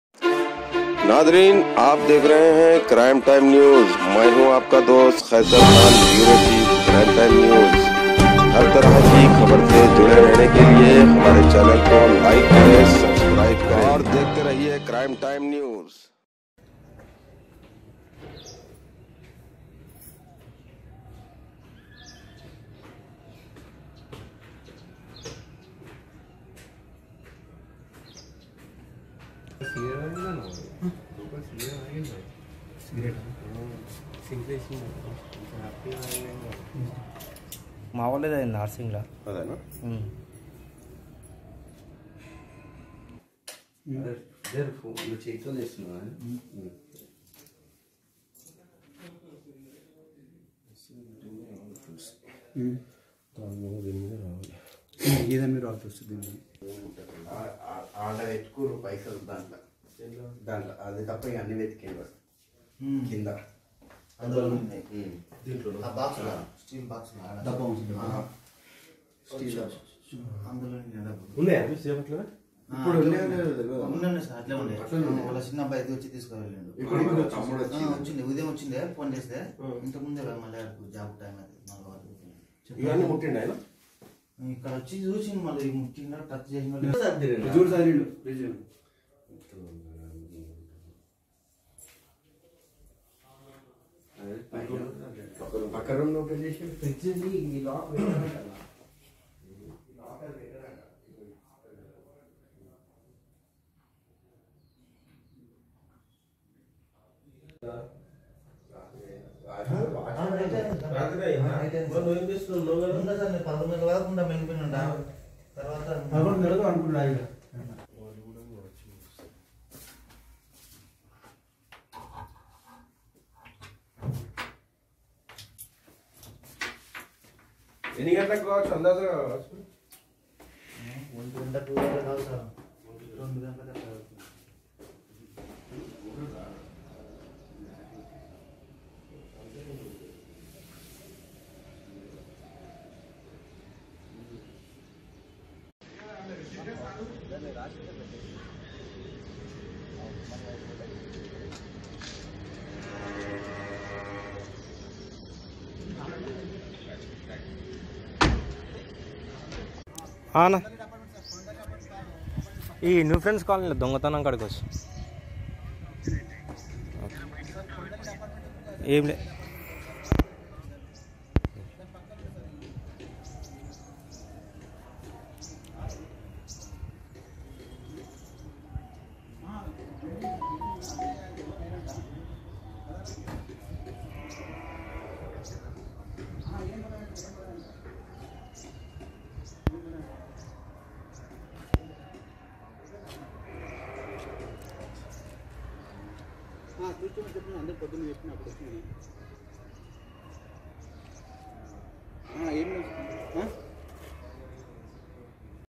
Home Burglary Took At Hyderguda Under Rajendra Nagar PS Limits
Home Burglary Took At Hyderguda Under Rajendra Nagar PS Limits Police Registered A Case And Investigation Underway.